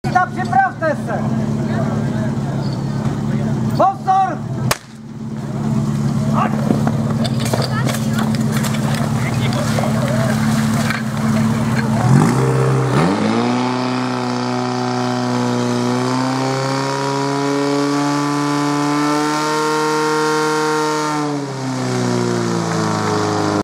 SDH Metylovice: Větřkovice muži v Jistebníku 2016
Jistebník, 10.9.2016, 28. ročník Memoriálu Josefa Konečného (muži) a 28. ročník O putovní pohár Zastupitelstva obce (ženy)